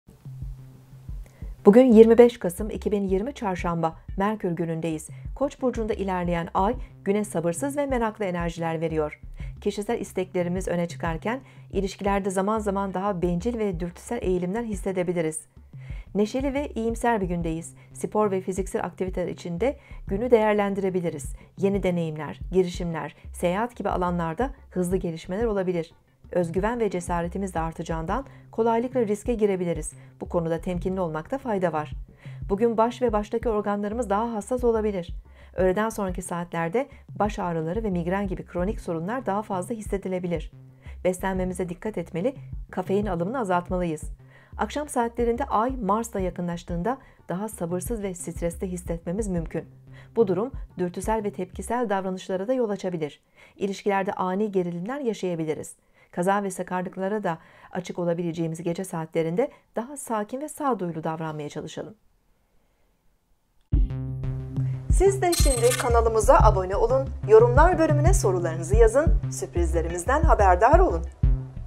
Günlük Burç Yorumları 25 Kasım 2020 Çarşamba (astroloji)
Günlük Burç Yorumu 25 Kasım 2020 Çarşamba (Tüm burçlar)
Bugün 25 Kasım 2020 Çarşamba, günlük burçlar ve astroloji yorumu (Günlük Burç Yorumları)